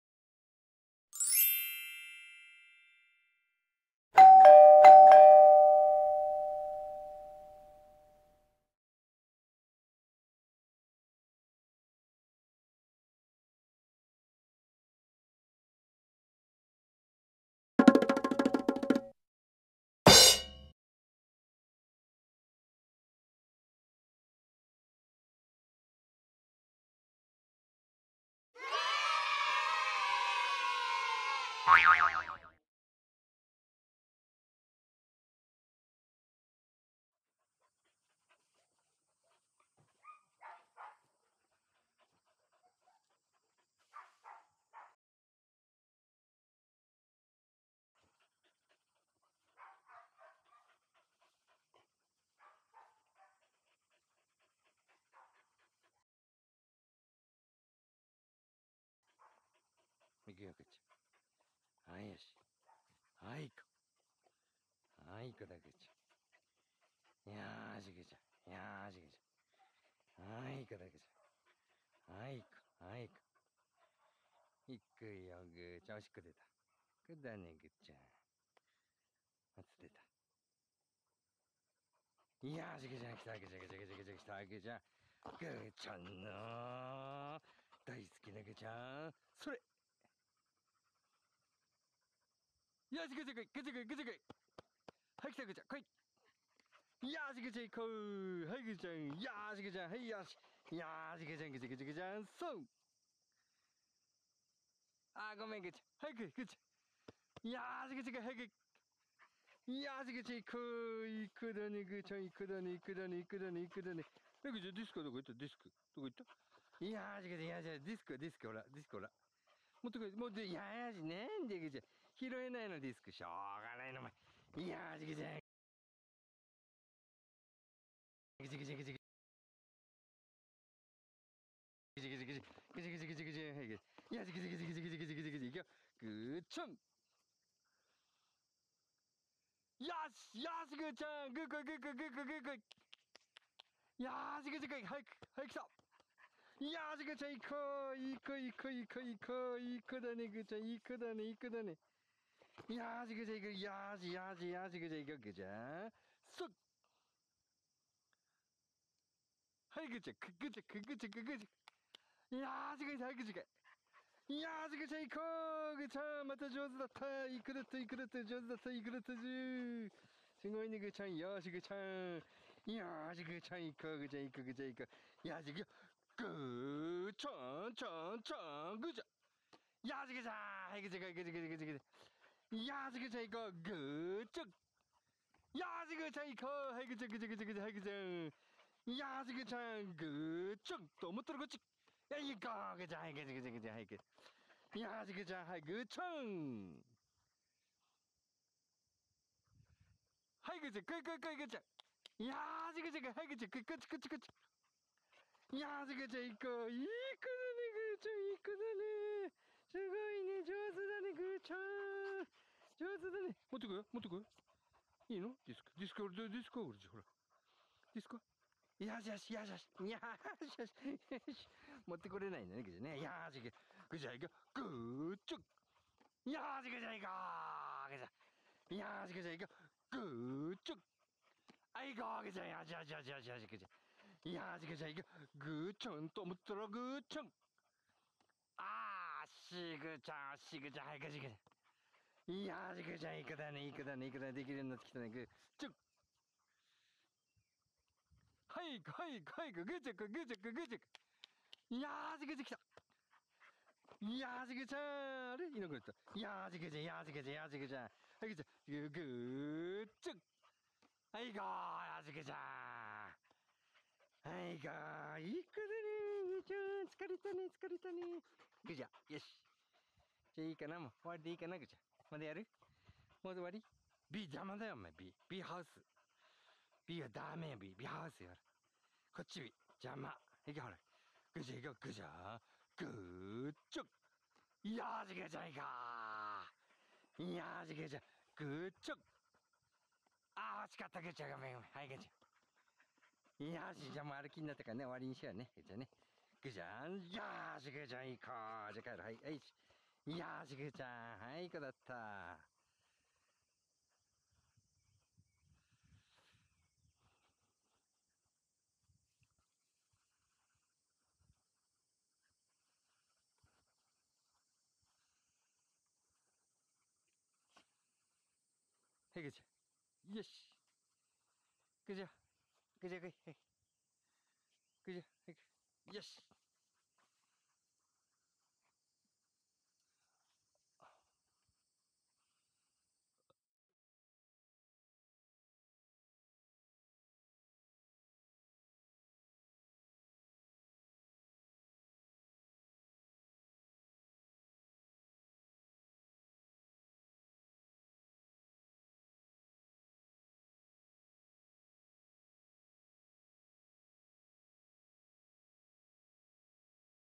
Labrador puppy 203days old♪ラブラドールレトリーバーの子犬・生後203日目・ブラック♀子犬グーちゃん.真夏の夕方レトリーブ練習動画♪だんだん上手になってきましたよ！
フィールド系ラブラドールレトリーバーの子犬を迎えました。
生後203日目のディスクトレーニング動画です。